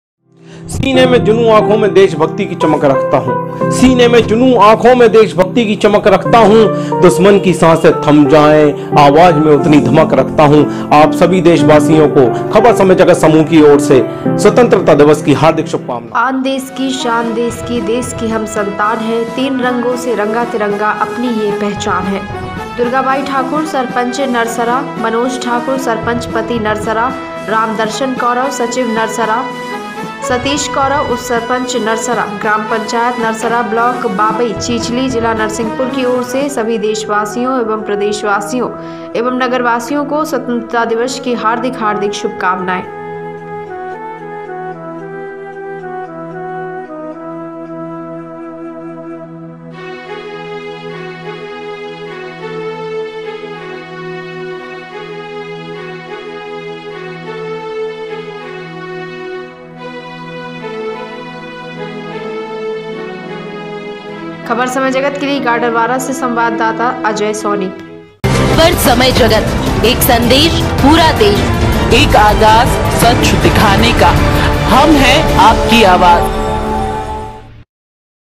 ग्राम पंचायत नरसरा ब्लॉक बाबई चीचली जिला नरसिंहपुर की ओर से स्वतंत्रता दिवस की हार्दिक शुभकामनाएं
गाडरवारा से संवाददाता अजय सोनी